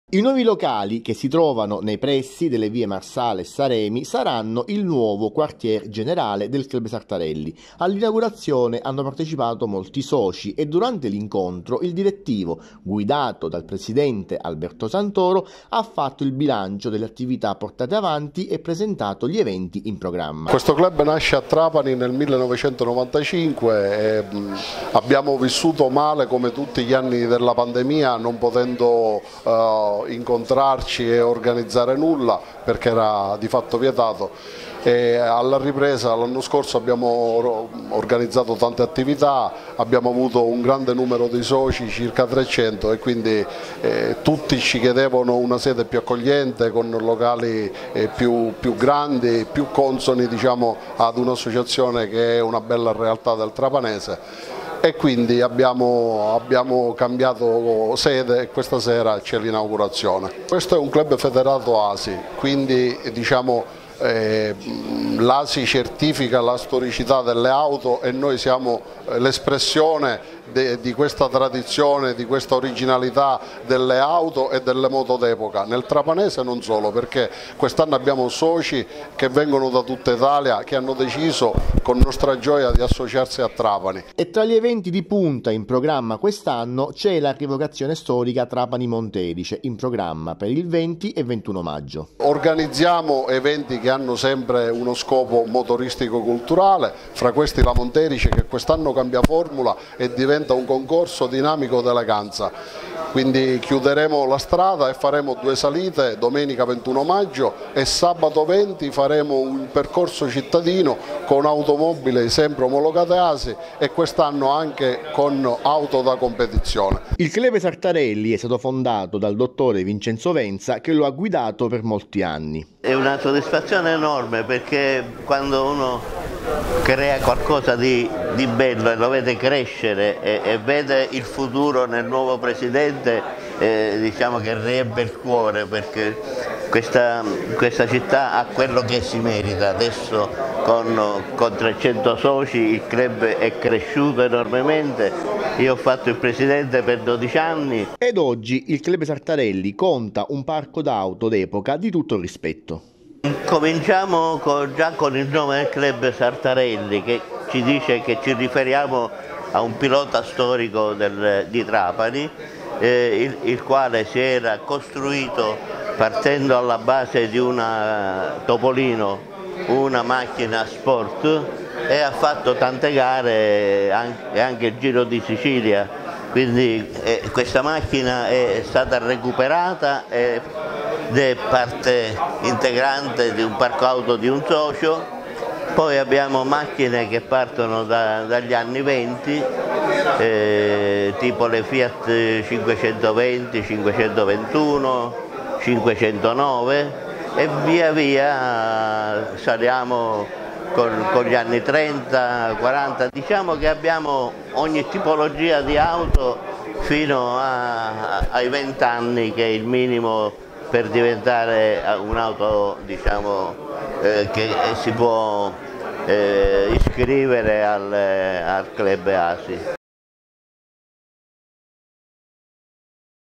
La nuova "Casa" del Club Sartarelli
Inaugurata ieri a Trapani, tra le vie Salvatore Bertino e Lorenzo Macaluso, la nuova sede del Club Sartarelli, associazione che si occupa di auto storiche organizzando diverse iniziative, tra cui la rievocazione della Trapani – Monte Erice.
I nuovi locali, che si trovano nei pressi delle vie Marsala e Salemi, saranno il nuovo quartiere generale del Club Sartarelli. All'inaugurazione hanno partecipato molti soci e durante l'incontro il direttivo, guidato dal presidente Alberto Santoro, ha fatto il bilancio delle attività portate avanti e presentato gli eventi in programma. Tra gli eventi di punta in programma nel 2023 c'è la rievocazione storica Trapani-Monte Erice, in programma per il 20 e 21 maggio. Il Club Sartarelli è stato fondato nel 1995 dal dottore Vincenzo Venza, che lo ha guidato per molti anni. Oggi un parco di auto d'epoca di tutto rispetto.